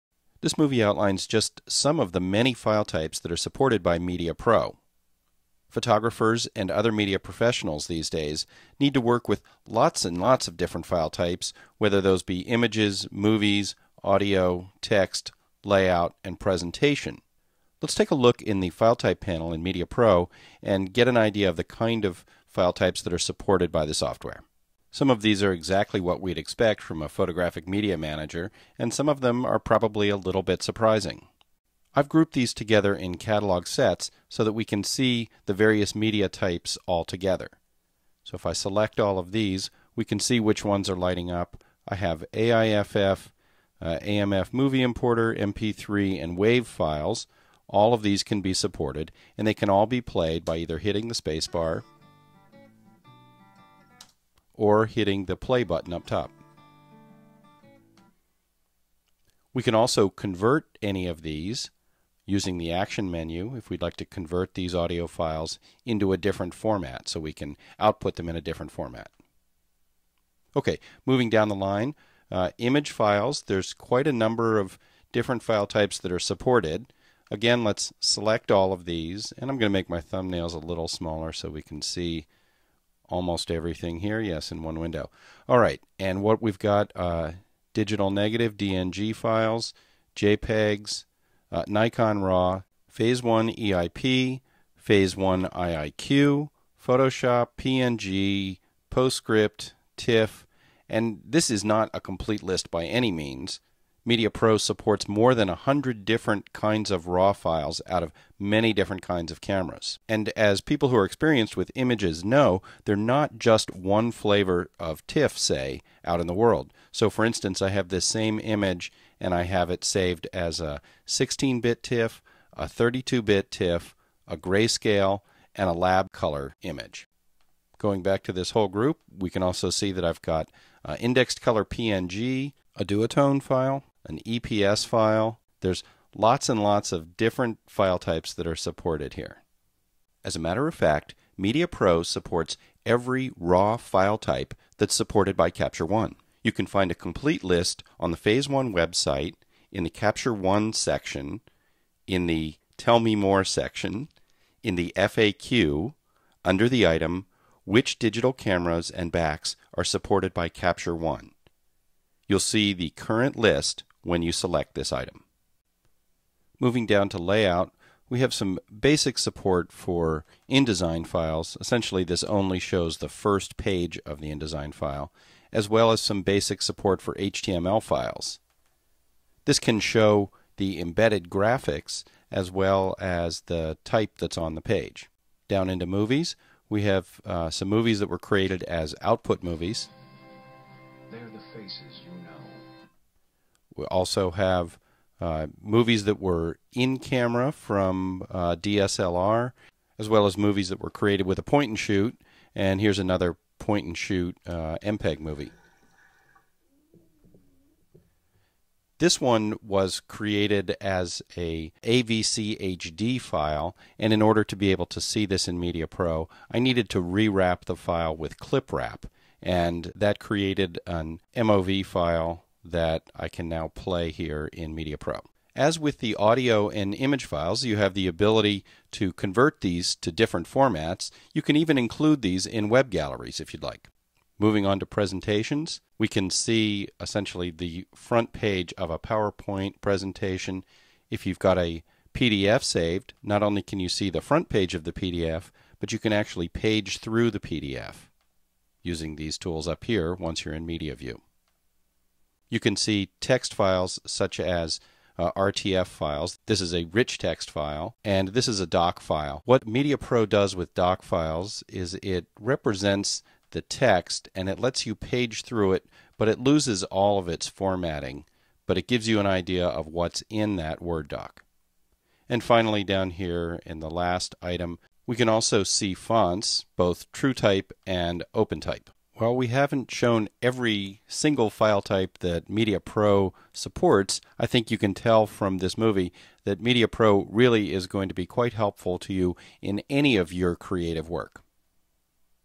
Supported file types | Media Pro Tutorial
Media Pro has very broad support of image files. It can read more than 100 different raw file types, supporting any raw file that is supported by Capture One. It can even catalog many file types that are not supported by Capture One, such as PNG files, images saved in Lab color, EPS files and Duotones.

Although Media Pro is primarily used to catalog photographic images, it is capable of indexing and even converting many additional classes of files. This includes the most popular formats of audio and video files, which can be organized, played in the catalog itself, and converted to derivative file types.  This capability makes Media Pro extremely useful for photographers and other media professionals who need to create, manage and make use of different media types. This video provides an overview of the file types that are supported by Media Pro.